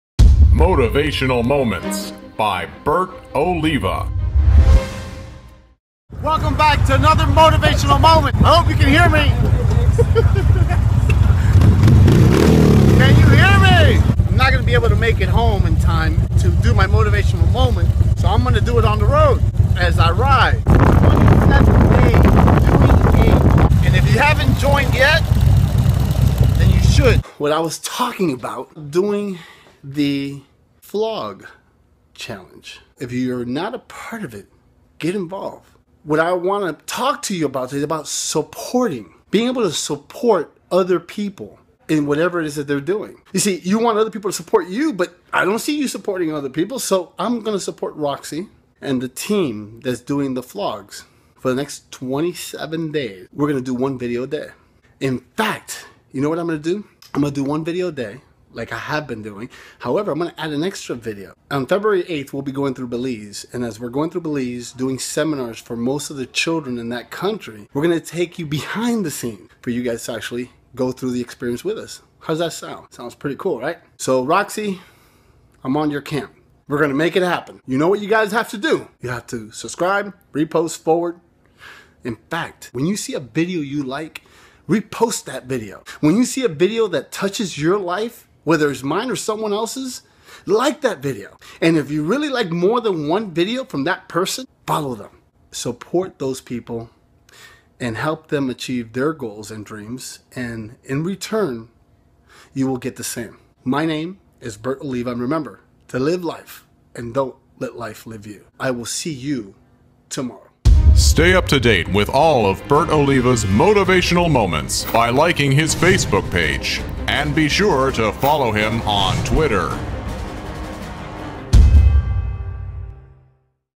SUPPORTING THE @THEROXIE CHALLENGE (BOMM 263)
We all need to take a moment now and then. Take a "Motivational Moment" with Bert Oliva and learn about SUPPORTING @THEROXIE CHALLENGE. Together, we can Make it Happen!

BOWA Studios
12080 Southwest 127 Avenue
Suite 305